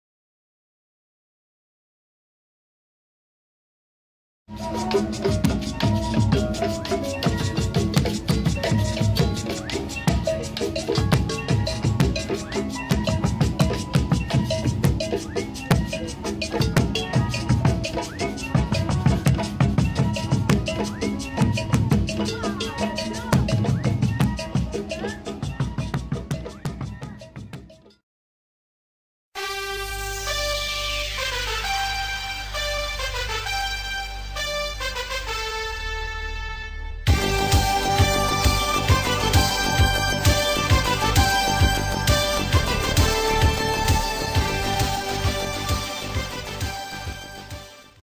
Entrez dans l'univers interstellaire - Enter the intestellar universe
Durant cette soirée intergalactique, partez à la rencontre de créatures étranges et exotiques venues d'une galaxie lointaine, très lointaine.

Meet some strange and exotic creatures coming from a galaxy far far away.